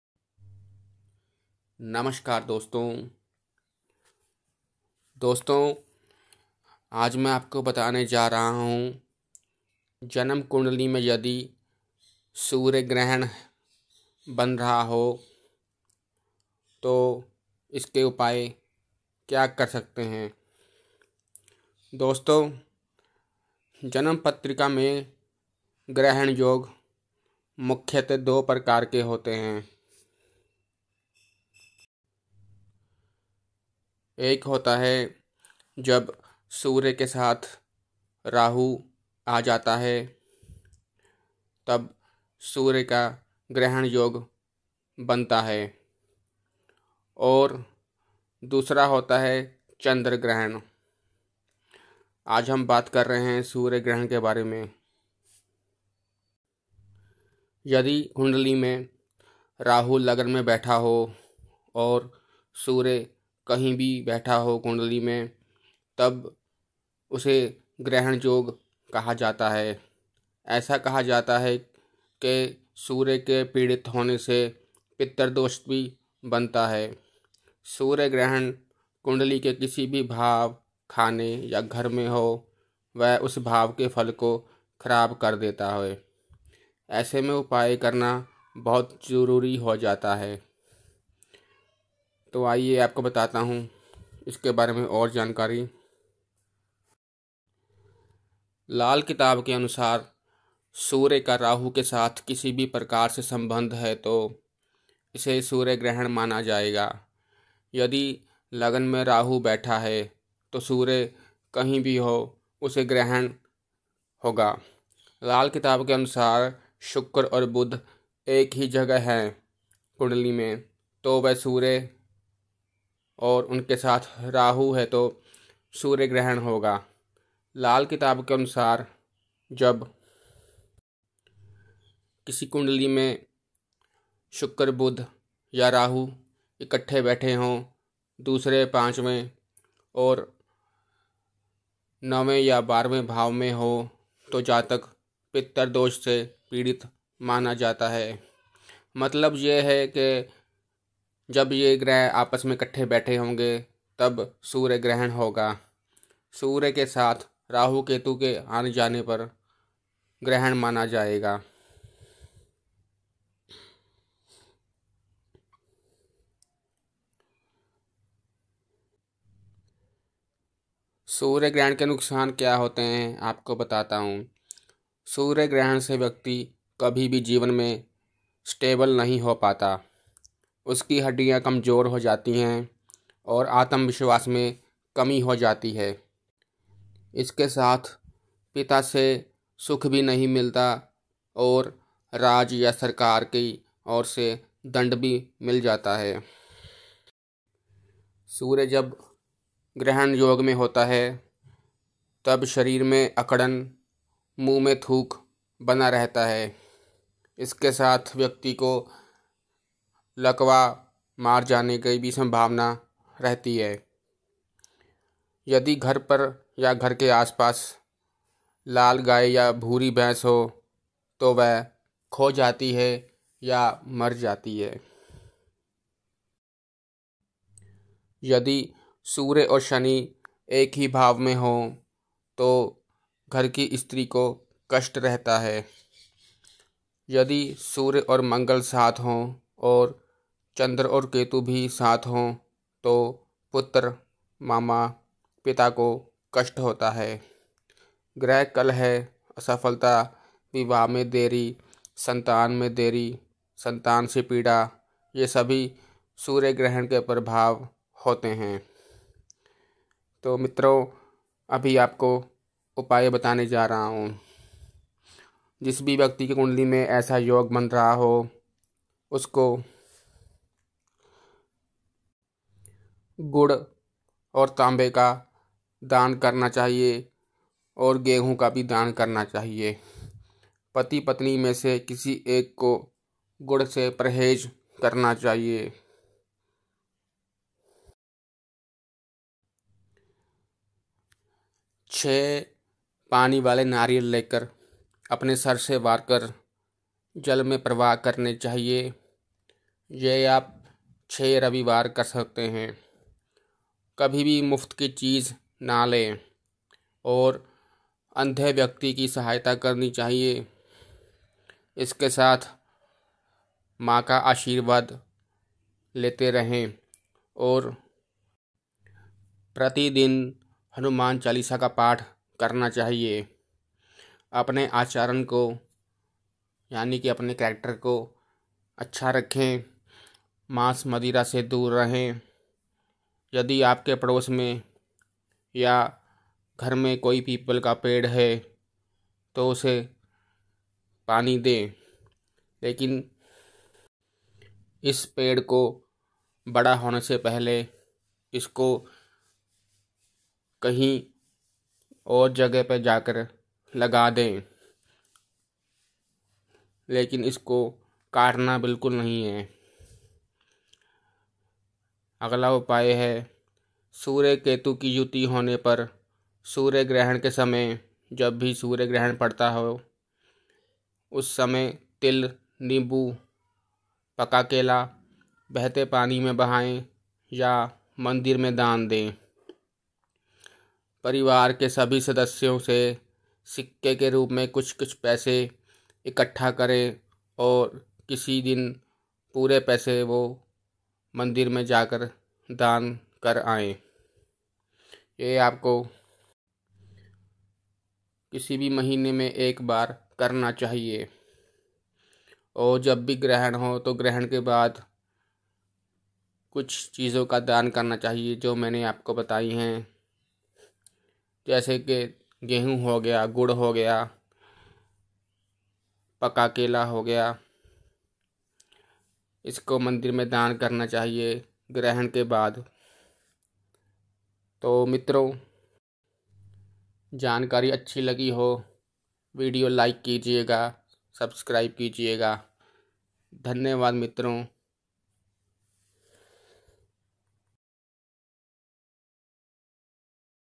कुण्डली में जब सूर्य ग्रहण हो तो यह उपाय करें
Knowledge about solllar eclipse in horoscope